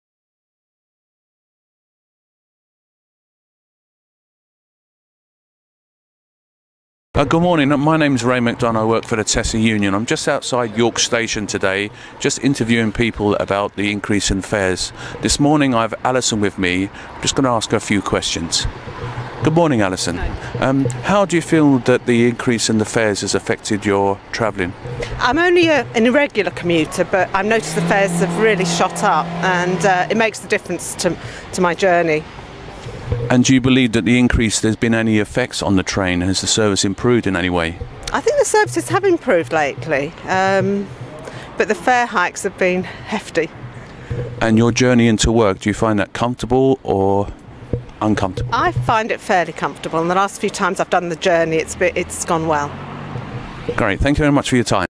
TSSA Film Group at York Station
Alison, a passenger at York Station, shares her views on the recent January 2013 train fare increases with TSSA Film Group member Ray McDonagh.